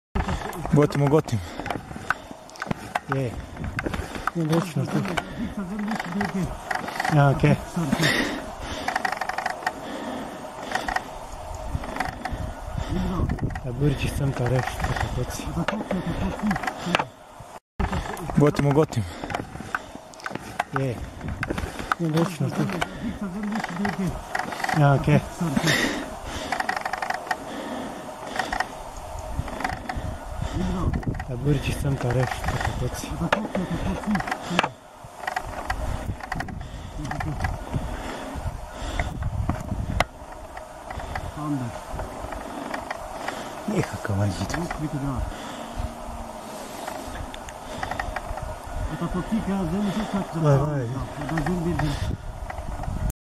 Leopard run away
This video is capture in Ghulkin Nature Conservation area.